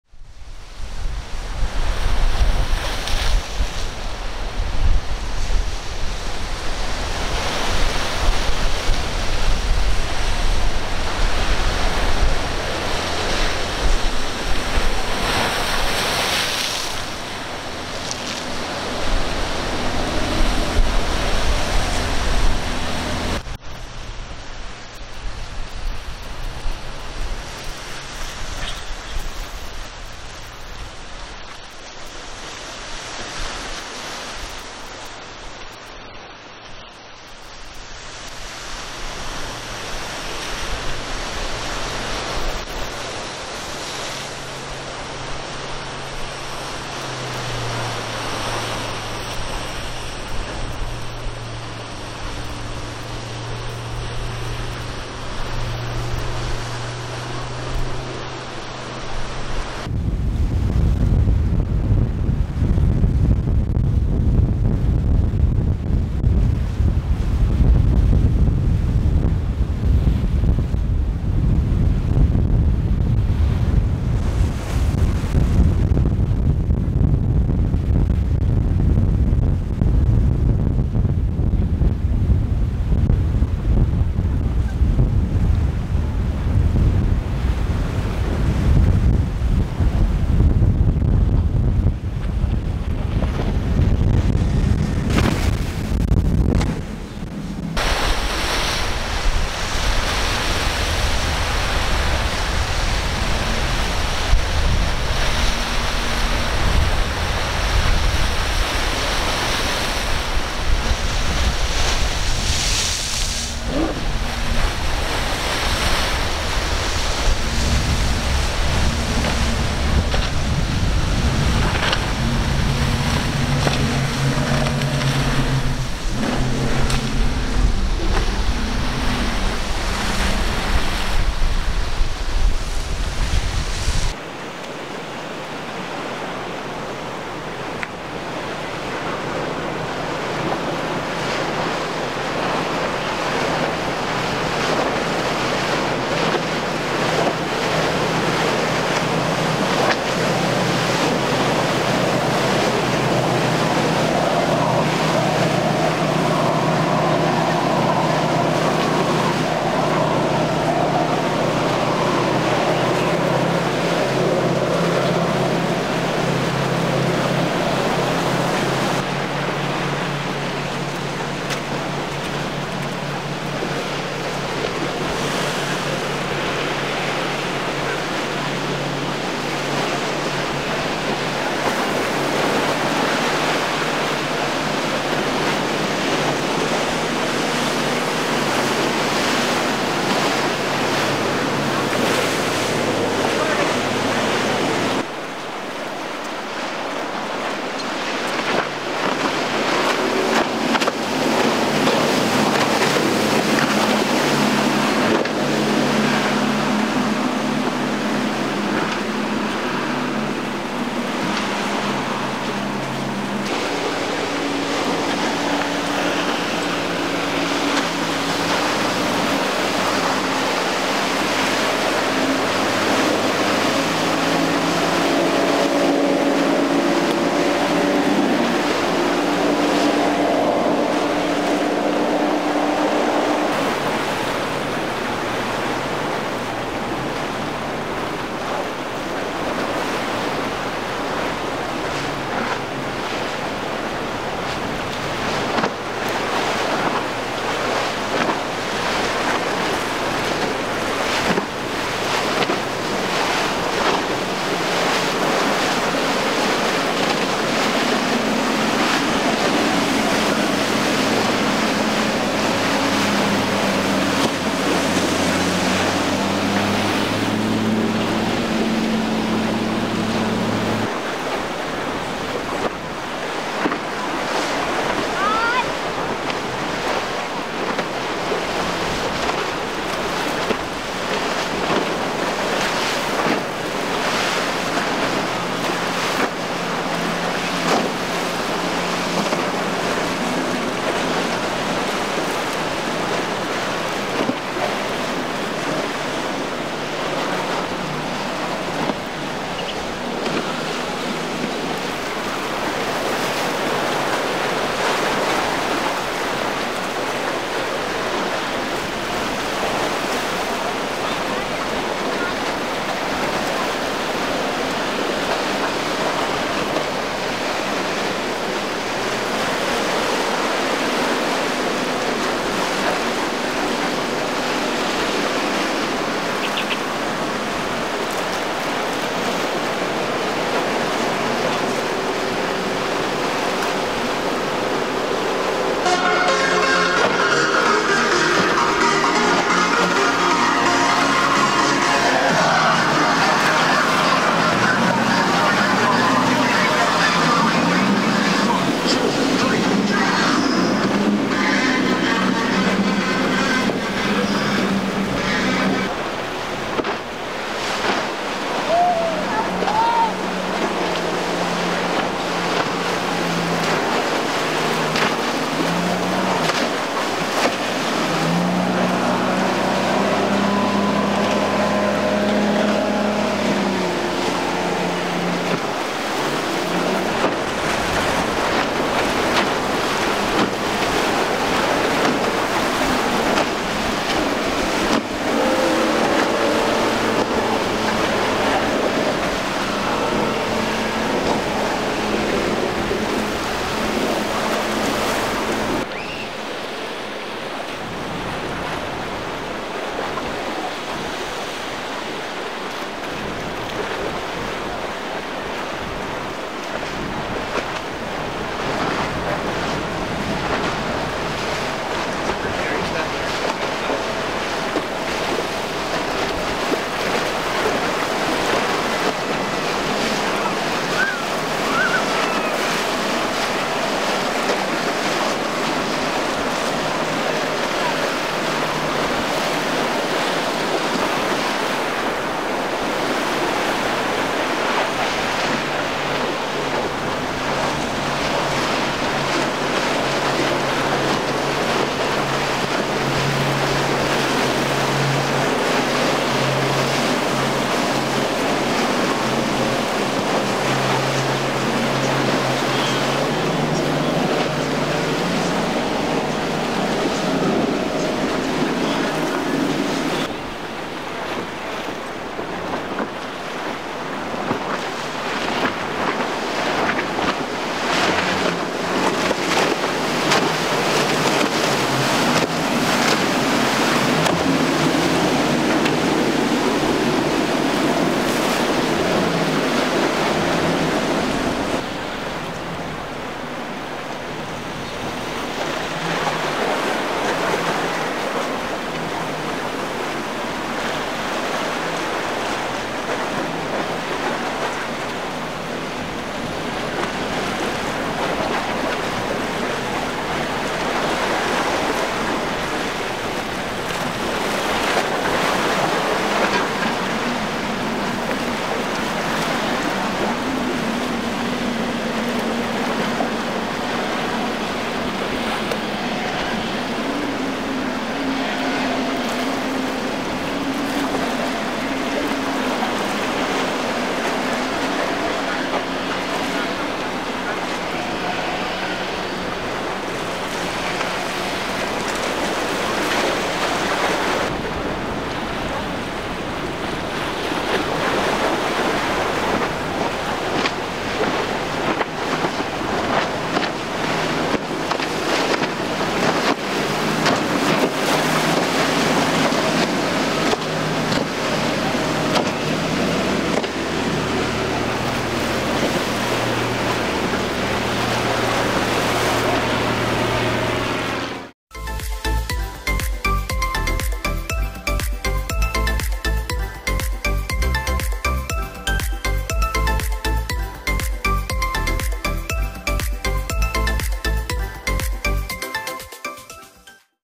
BOAT LIFE: CAPTAIN FULL THROTTLE THROUGH ROUGH SEAS | BOCA INLET| JUPITER INLET
CAPTAIN FULL THROTTLE THROUGH  ROUGH SEAS | BOCA INLET | JUPITER INLET | BOAT LIFE

In this episode we bringing you guys great content from both Boca and Jupiter inlet. We still got pretty windy days down here in the south which makes it a bit more challenging for the captains.

As always, thank you for visiting my channel! Lot more great content on the way!

This video was created for educational and entertaining purpose.